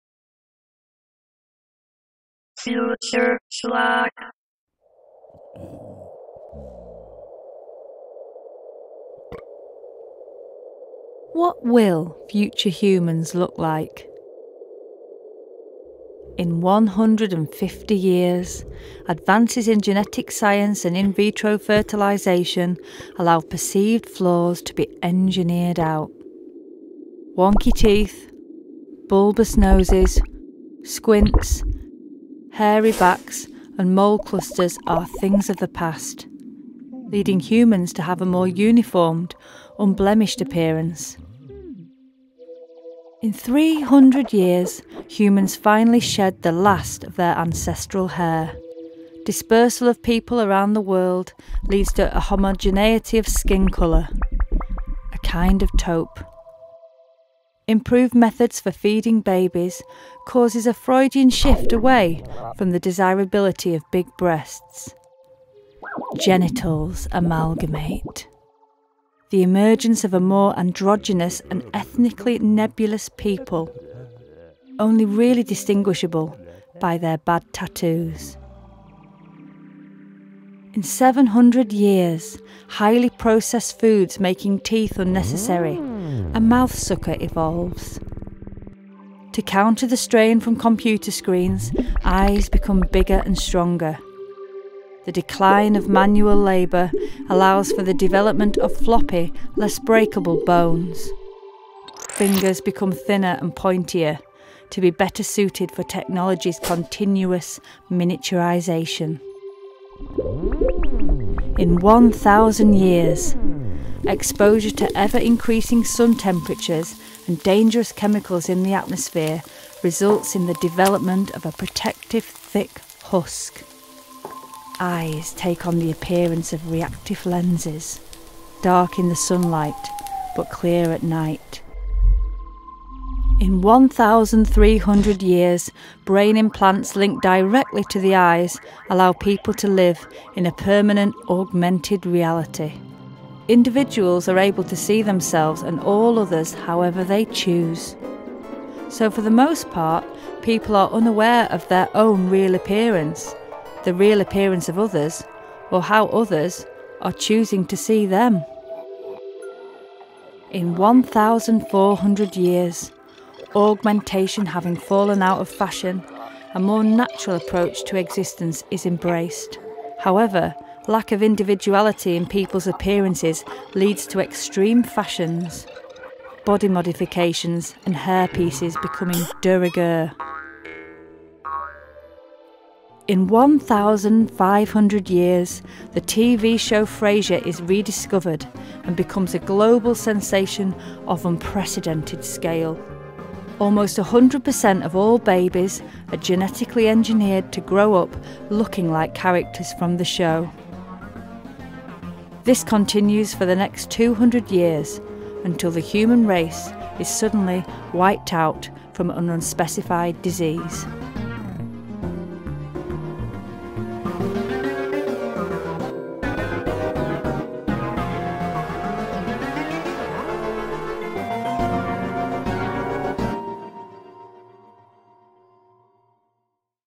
What Will Future Humans Look Like?
A short animation about the evolution of humans.
Written and animated by Casey Raymond
Narrated by Holly Blackwell